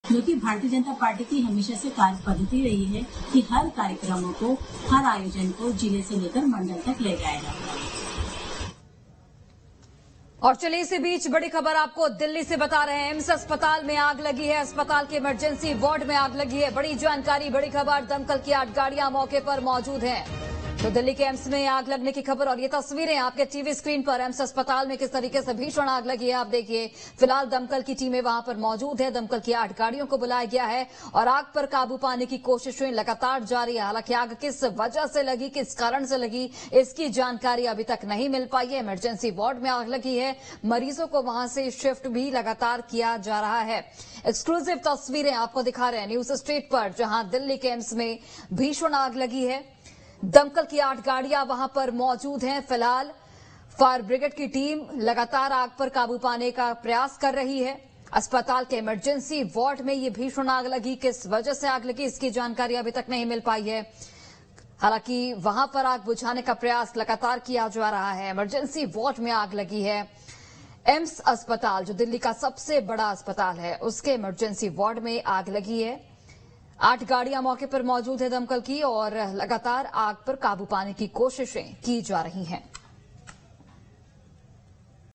Delhi AIIMS अस्पताल में लगी भीषण आग, दमकल की 8 गाड़ियां मौके पर मौजूद | News State MP CG
दिल्ली से बड़ी खबर.. AIIMS अस्पताल में लगी आग.. अस्पताल के इमरजेंसी वार्ड में लगी आग.. दमकल की 8 गाड़ियां मौके पर मौजूद

Big news from Delhi.. Fire broke out in AIIMS hospital.. Fire broke out in the emergency ward of the hospital.. 8 fire tenders present on the spot.